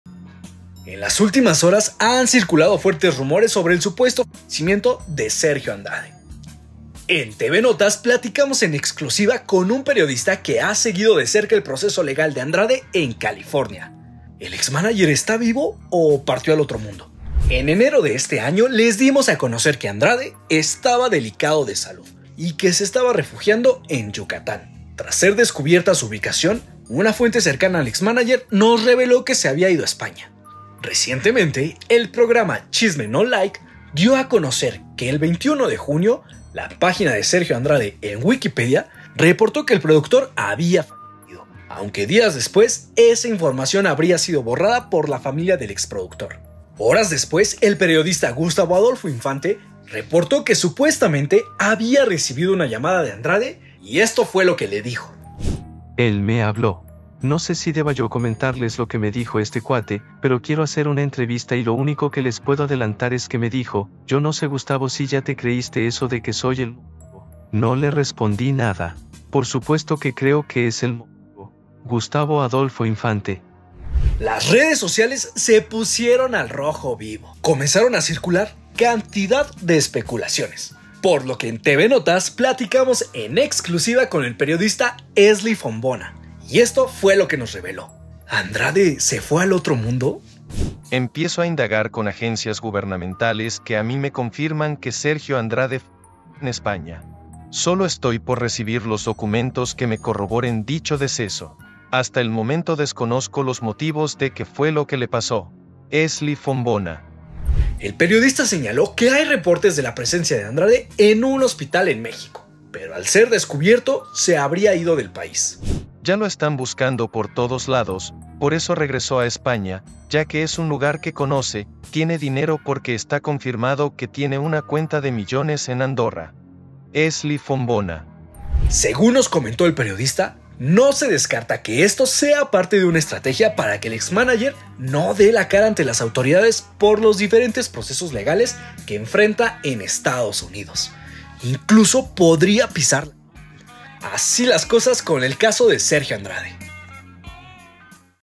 ¿Sergio Andrade fallecio?
En las últimas horas, han circulado fuertes rumores sobre el supuesto fallecimiento de Andrade. En TVNotas, platicamos en exclusiva con un periodista que ha seguido de cerca el proceso l3gal de Sergio en California. ¿El exmánager está vivo o partió al otro mundo?